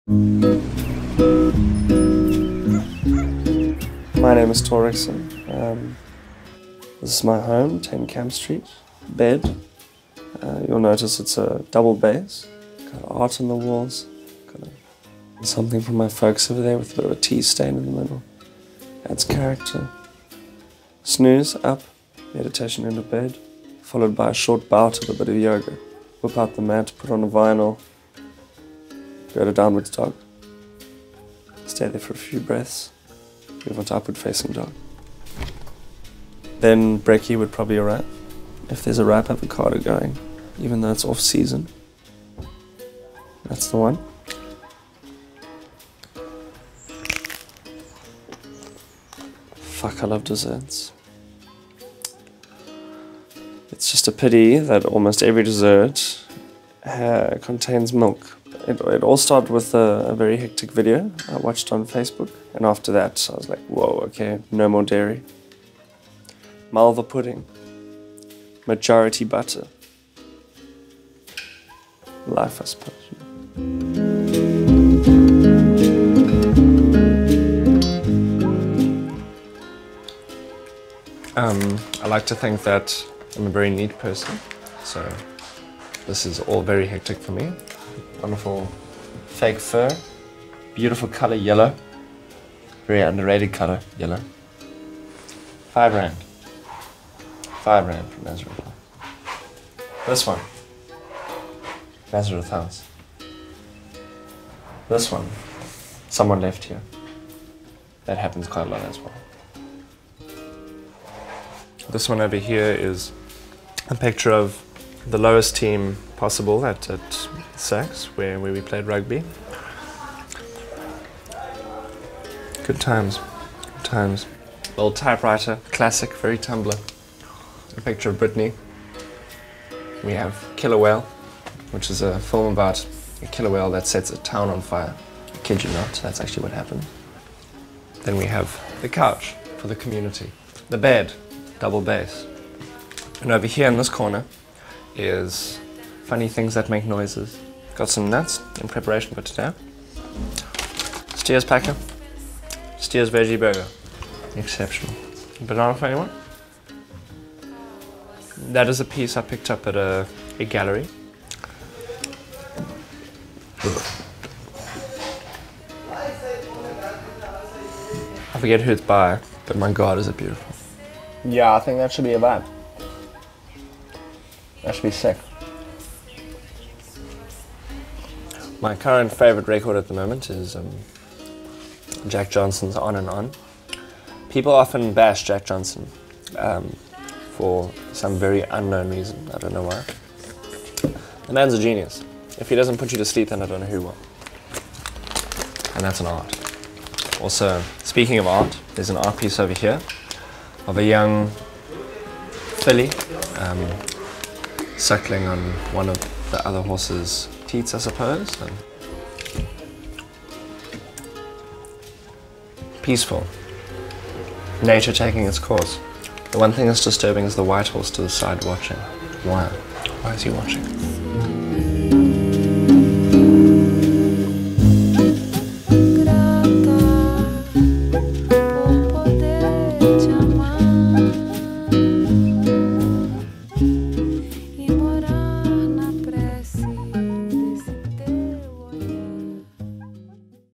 "DON'T ASK ME ABOUT MUSIC" Feat. Thor Rixon - PART 1
Part 1 of 2 of our new web series "Don't Ask Me About" where we document the lives of South African artists, avoiding the expected questions and getting a unique insight in to their day to day life.

Directed and Edited by Rob Smith
Produced by Rob Smith and William Nicholson
Cinematography by Pierre De Villiers
Grade by Paul Speirs
Sound Mixing by Chris de Wet Bornman
Music Supervised by Viktorija Veilentaite

Got a suggestion of who should be featured next on "Don't Ask me About?"
They don't necessarily need to be involved in music, but any creative sphere.